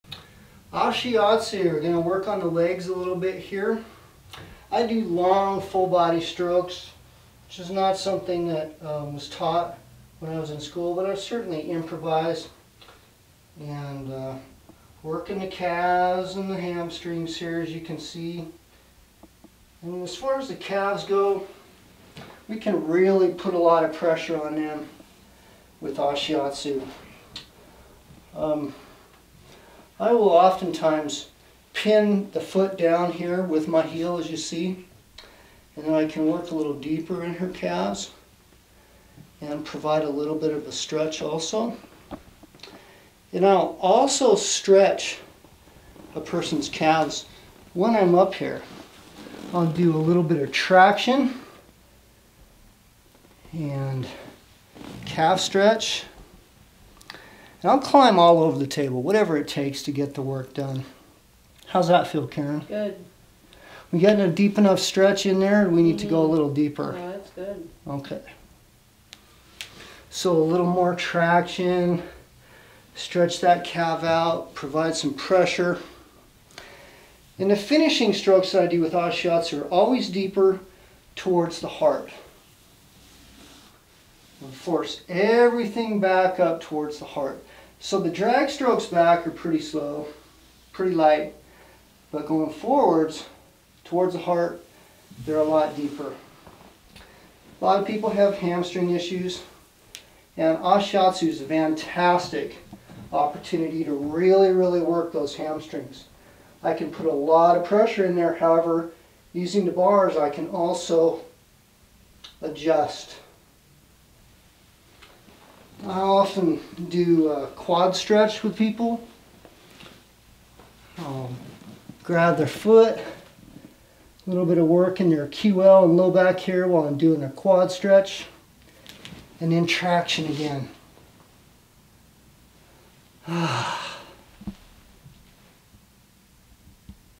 Ashiatsu deep tissue massage, Kalispell Mt., Johnny Cochrane LMT
I have a massage therapy practice in Kalispell, sessions are available by appointment only.